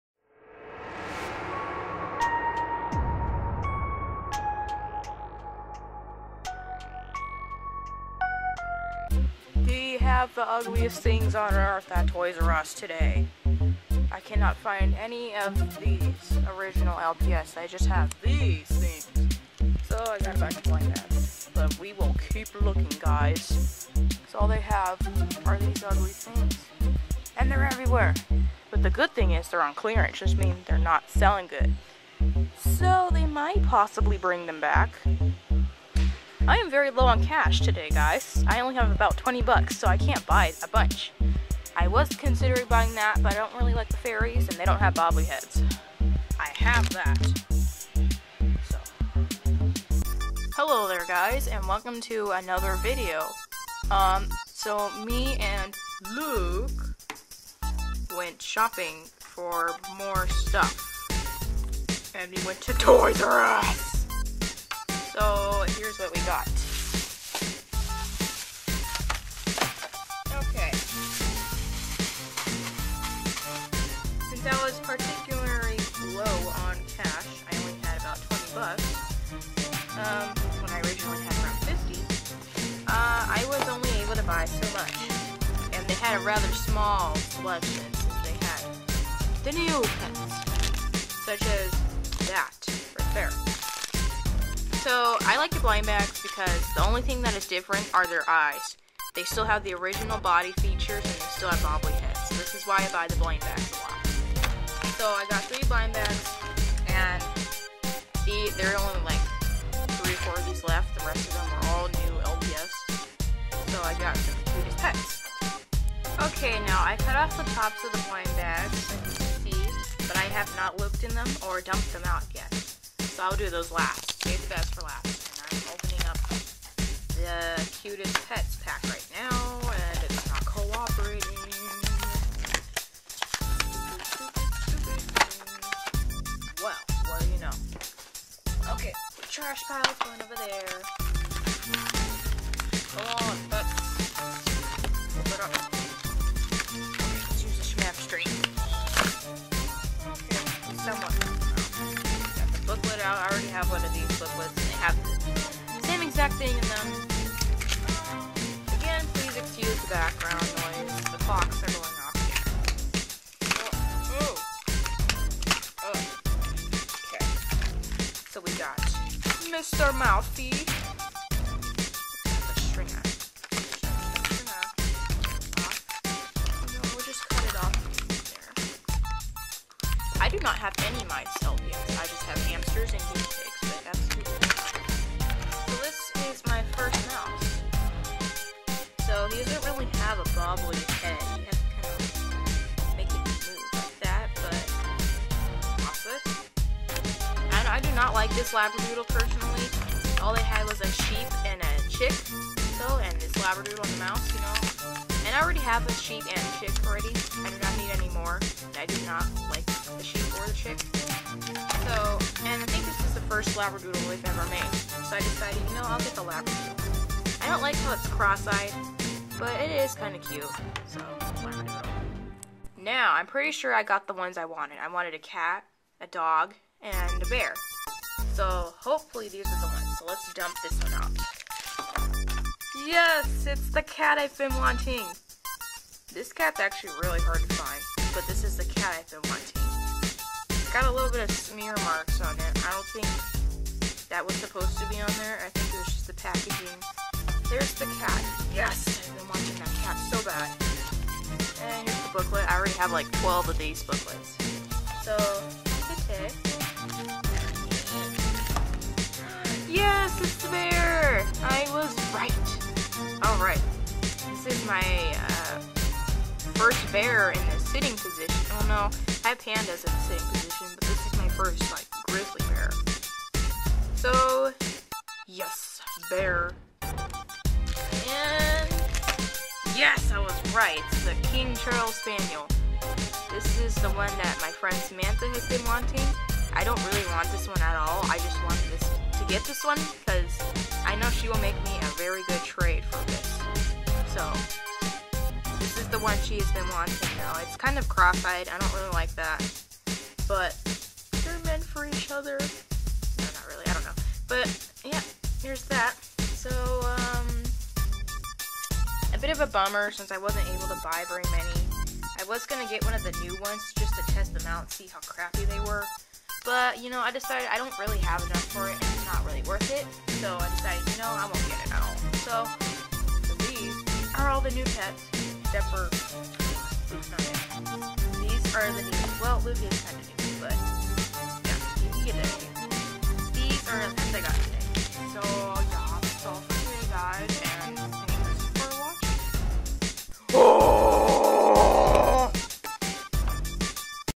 LPS: Shmev Unboxing!
i went to "toys r us" for some lps but it was a bummer :(